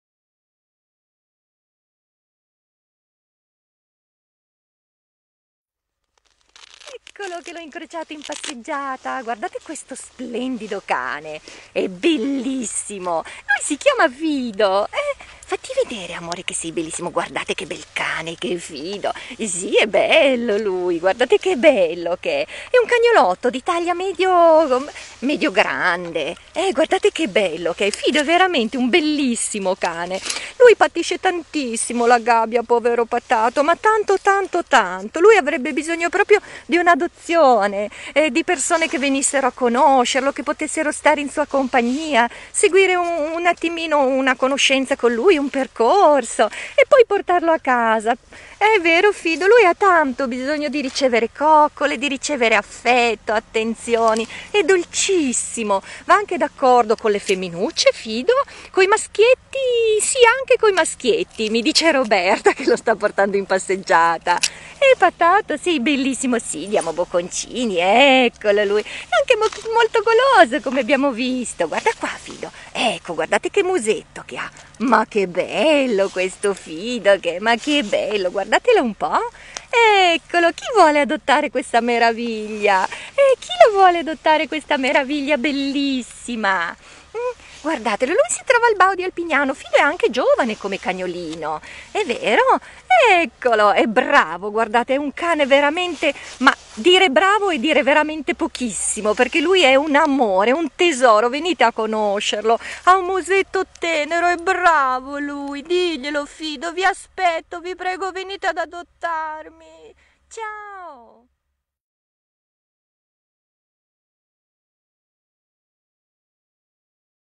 Fido
UNA CONDIVISIONE PER FIDO NON ACCETTA PIU' LA RECLUSIONE IN GABBIA , VUOLE LASCIARSI MORIRE ...AIUTATECI PER FAVORE ...! Fido: uno sguardo triste che ti colpisce al cuore .. !.. Fido è stato preso da cucciolo ed è vissuto con 2 anziane volpinette in una famiglia dove non ha ricevuto regole ne educazione .. ha trascorso 2 anni relegato in casa e su un balcone , mai una passeggiata o una uscita al guinzaglio .. Fido ha iniziato quindi a dare segnali di grande stress mordendosi la coda e le zampe . . l’anziana proprietaria non essendo in grado di gestire il problema ci ha chiesto aiuto e abbiamo deciso di accoglierlo al Bau .. piano piano Fido ha imparato a socializzare con le persone , a scoprire quanto sono divertenti le passeggiate , come è bello correre su un prato e quanti odori interessanti ci sono , a conoscere la dolcezza delle coccole e la gioia di ricevere attenzioni .. Fido è un cagnolino con tanta voglia di vivere , affettuoso e buono .. per lui tutto è una scoperta .. Fido ora è pronto per una nuova adozione , venite a conoscerlo ..!.. si trova al Rifugio Bau di Alpignano (TO) e verrà dato in adozione in Piemonte vaccinato e microcippato con controlli pre e post affido .. per info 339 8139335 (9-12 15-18) oppure